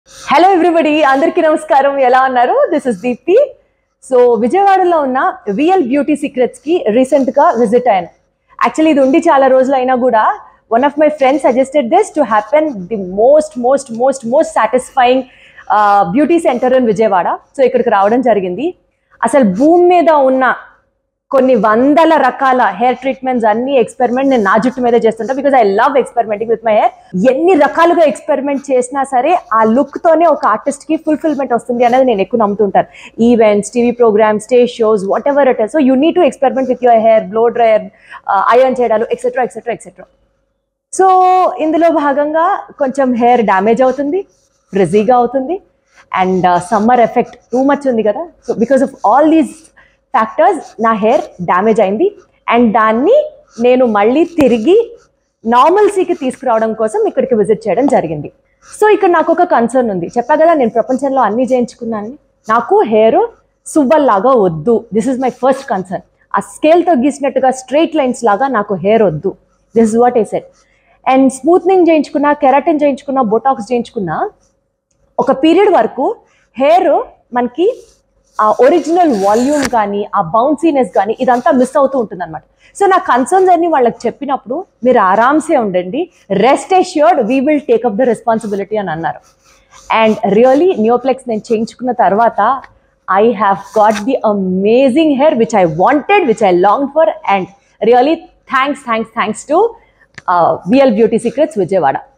Are you facing Hair Issues choose VL beauty Secrets || deeptinallamothu || neoplex
I get rid of frizzyness and dryness of my hair from Vl beauty secrets advanced Neoplex hair treatement , Neoplex Hair Treatment promises to exceed your expectations and leave you with hair that turns heads wherever you go."🥰😍

vl beauty secrets
deeptinallamothu
botox
neoplex
vl beauty secrets near vijayawada